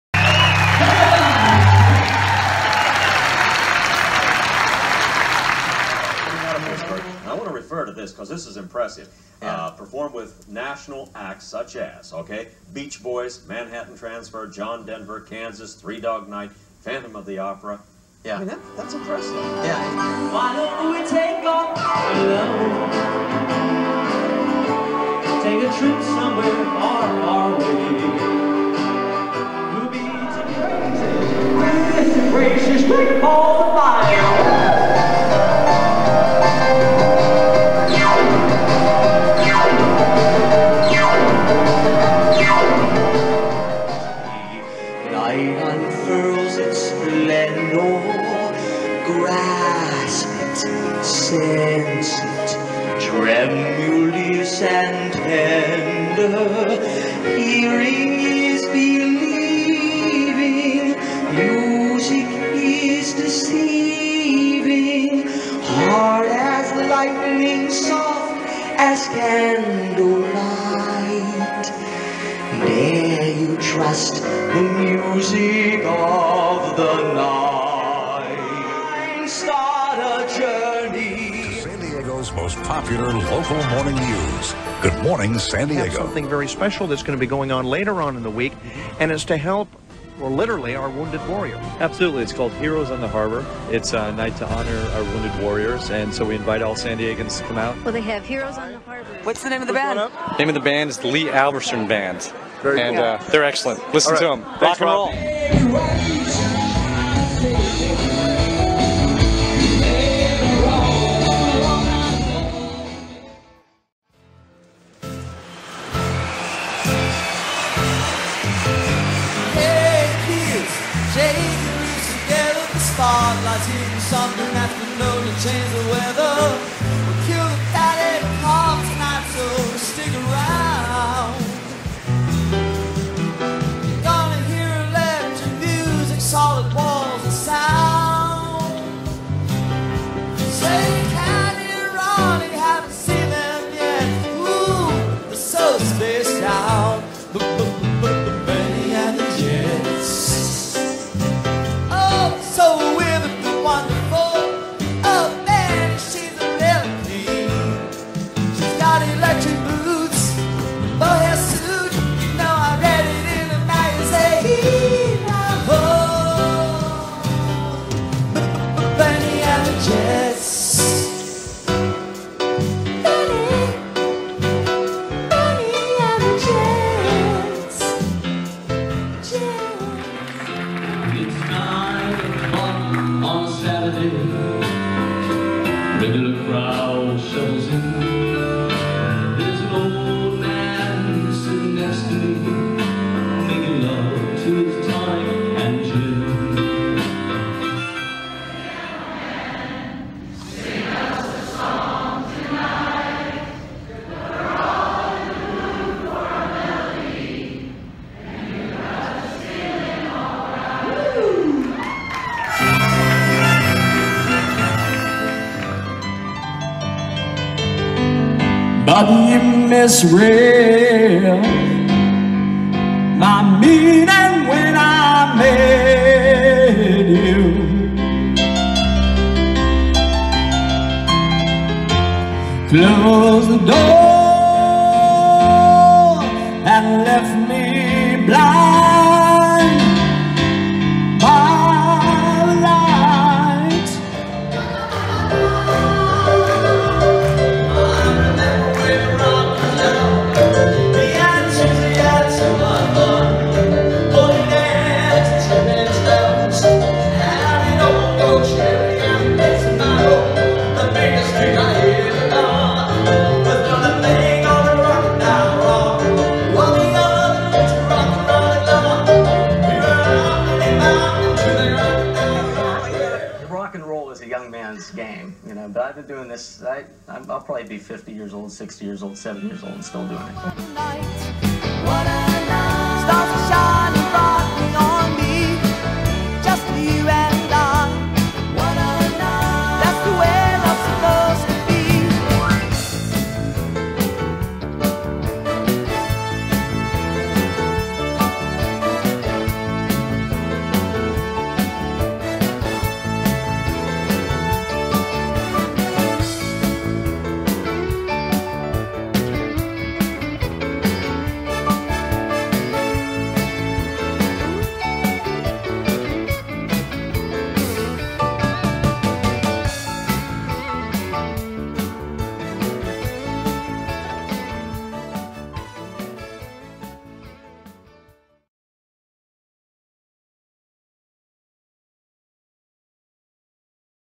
Lee Alverson Retrospective 2022
overview of Award winning Performing artist Lee Alverson's career